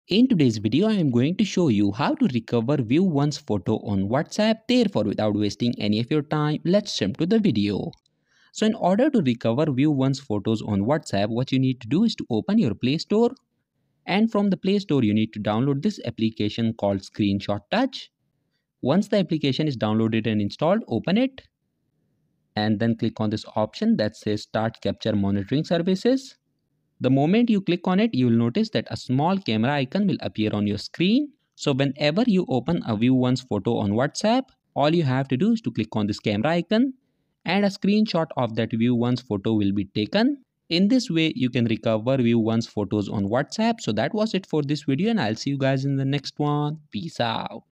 How to Recover WhatsApp View Once Photos (2023) | How to Replay View Once Photos on WhatsApp
Do you want to recover a view once photo on whatsapp in 2023? When we receive a view once photo or video on whatsapp we are only able to see it one time before it expires. So in this video i will show you how to replay view once photos on whatsapp multiple times.